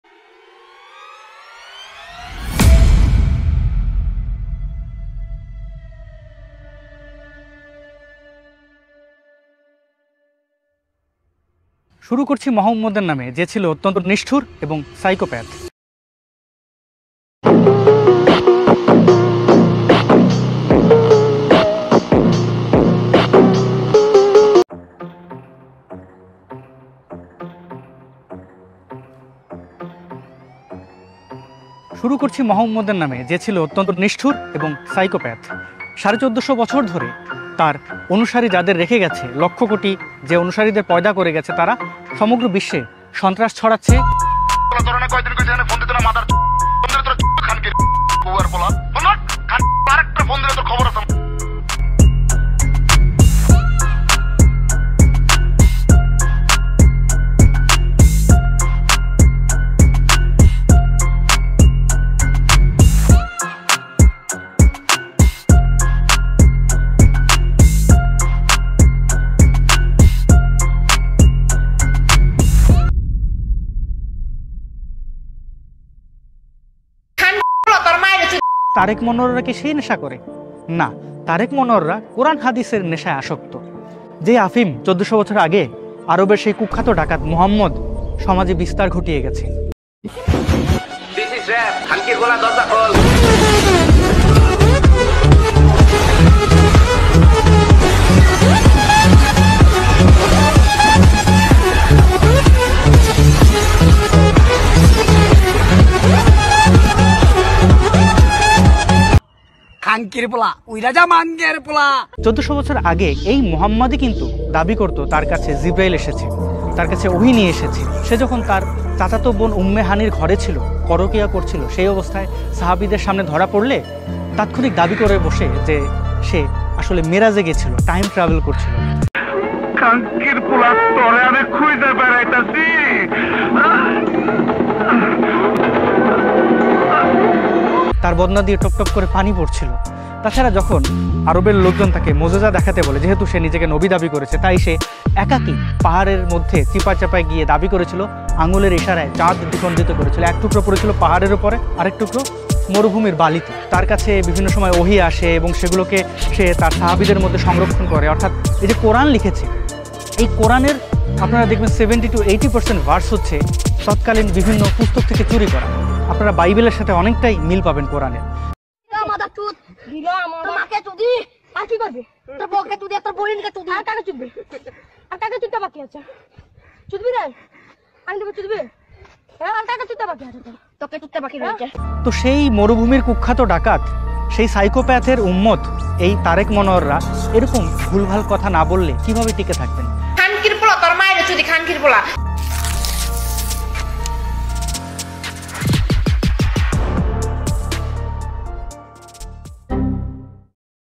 Asad Noor The Real Mother****er | Najmusiyan
The video contains a lot of profanity so watch at your own risk.  Young children stay away.  After watching Asad Noor's video, my inner reaction was exactly like this.  So the video is made with my personal feelings.  I respectfully inform the people of other religions that today you are laughing at my religion because he is an atheist and he don't believe in our God and don't believe in your Tagore either.